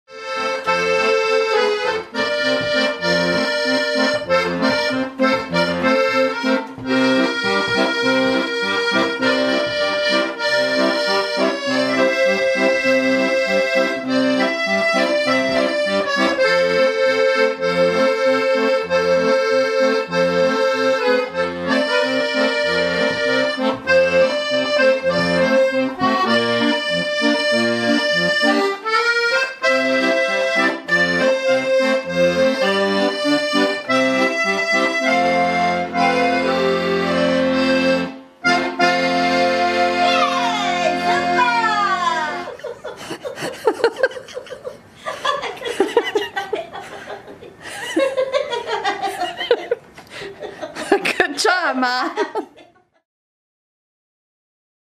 Hairless dog hilariously dances along to grandma playing the accordion
Adopted from the Bald Is Beautiful Hairless And Small Breed Rescue by a couple of ladies who absolutely adore him, Nathan totally makes up in entertainment what he lacks in hair.